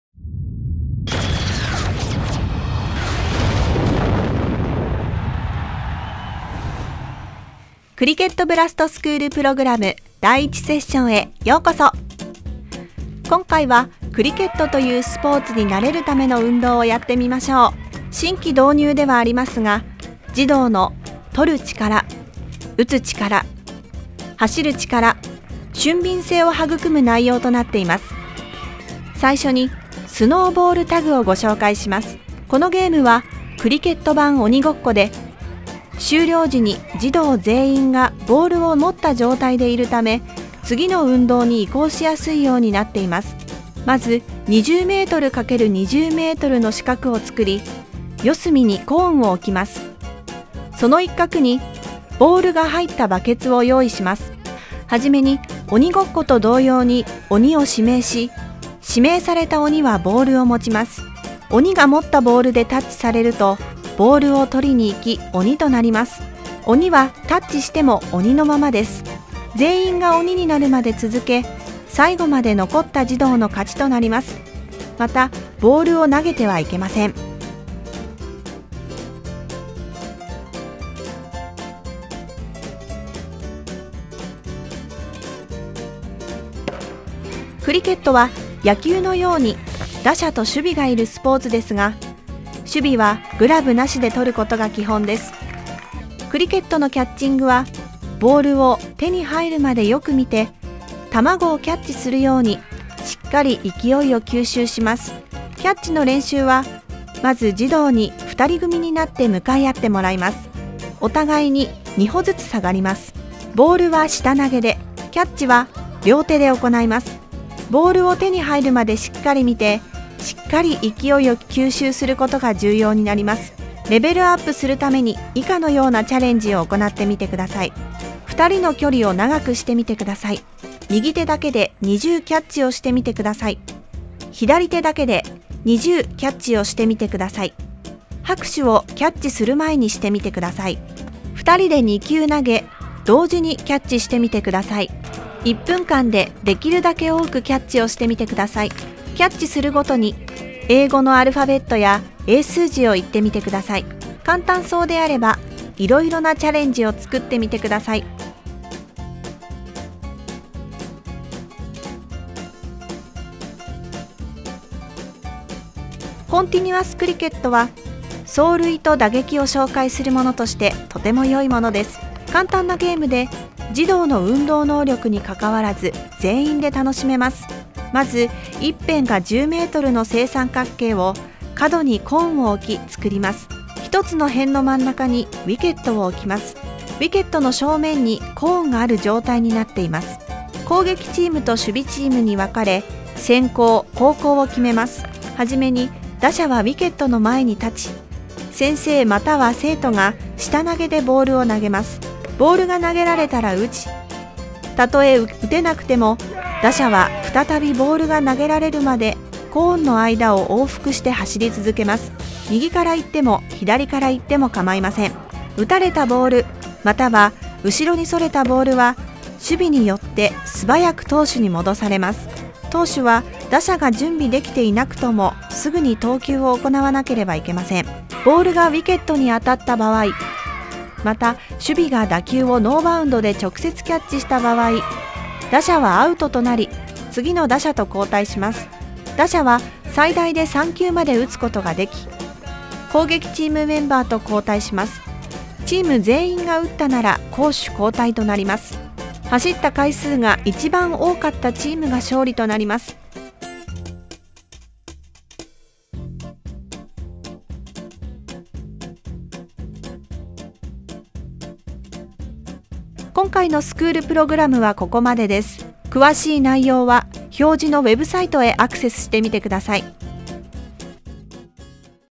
CRICKET BLAST スクールプログラム セッション1
児童のランニング、バッティング、キャッチング、俊敏性を向上させることのできるクリケットの基本練習を紹介いたします。
・スノーボールタグ
・コンティニュアス クリケット(パート1)